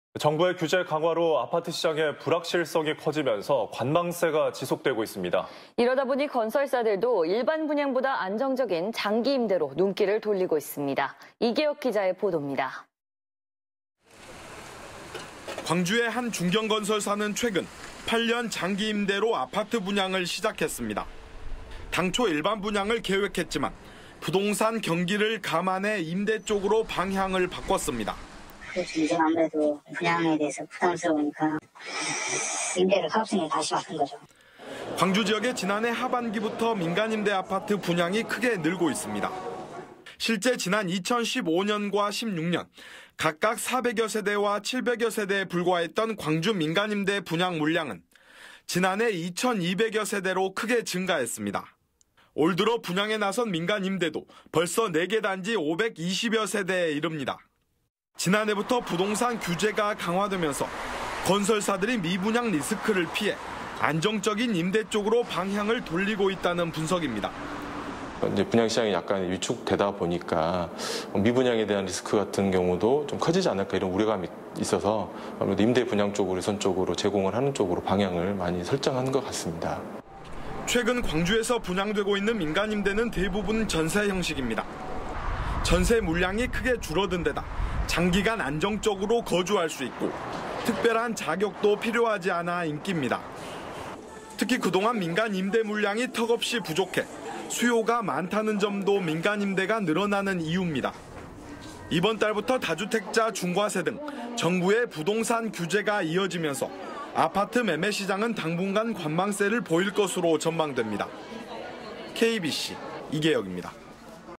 시장 불확실성 여파..장기 임대 아파트 분양 급증
정부의 규제 강화로 아파트 시장의 불확실성이 커지면서 관망세가 지속되고 있습니다. 이러다보니 건설사들도 일반 분양보다 안정적인 장기 임대로 눈길을 돌리고 있습니다.

이계혁 기자의 보도입니다.